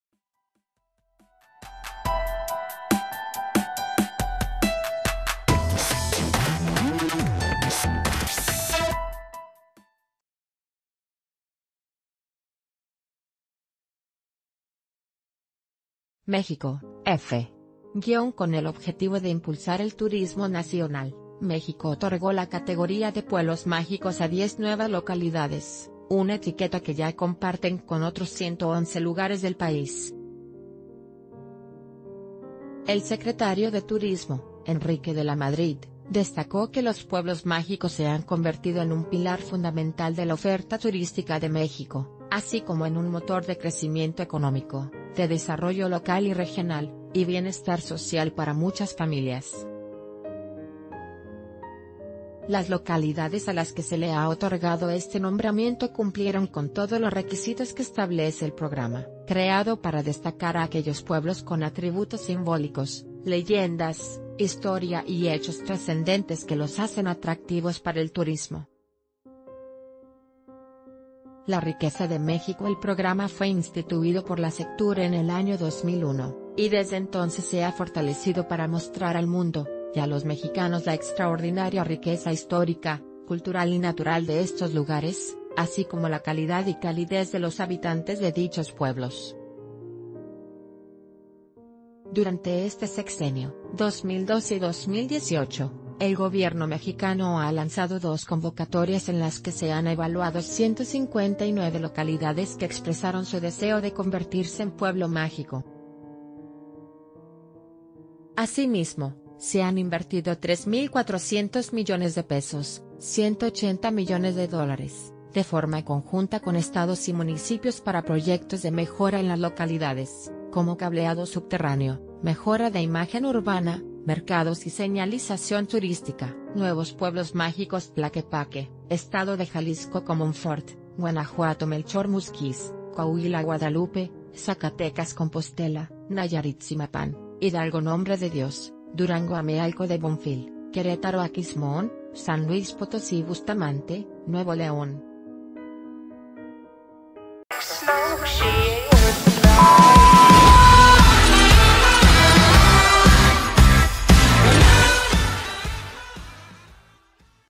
Diez nuevos “pueblos mágicos” en México - El Diario de Yucatán
MÉXICO (EFE).- Con el objetivo de impulsar el turismo nacional, México otorgó la categoría de “pueblos mágicos” a 10 nuevas localidades, una etiqueta que ya comparten con otros 111 lugares del país. El secretario de Turismo, Enrique de la Madrid, destacó que los “pueblos mágicos” se han convertido e...